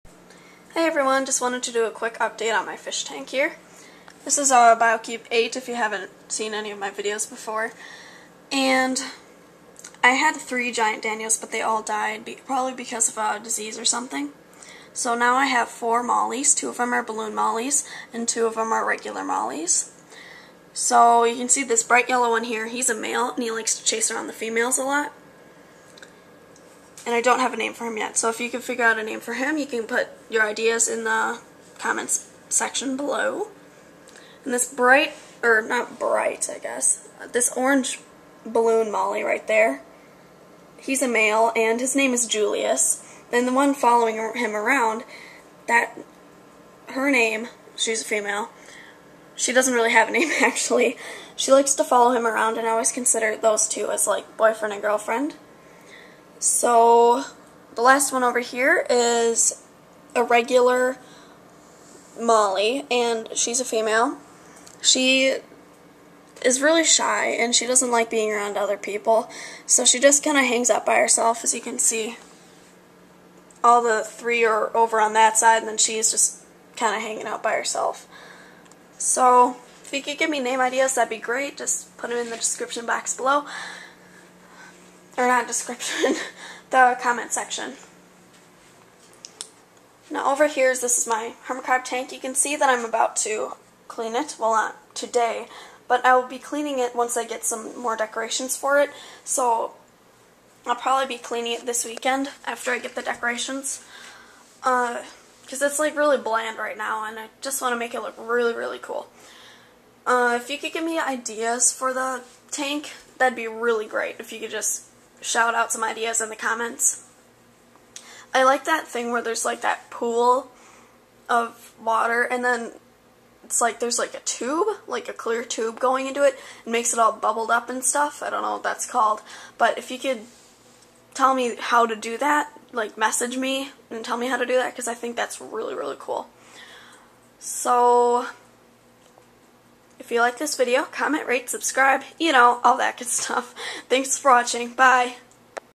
Update on my fish Tank! (6/20/11)
Got some new fish! If you have any ideas for names, post them in the comments below! If you have any hermit crab tank ideas, post that in the comments as well! Thanks!

ICFROGS